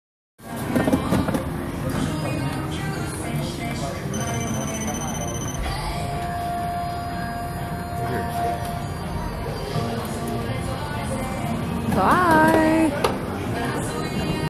Bullet Train Sushi
Special orders are brought to you on a bullet train.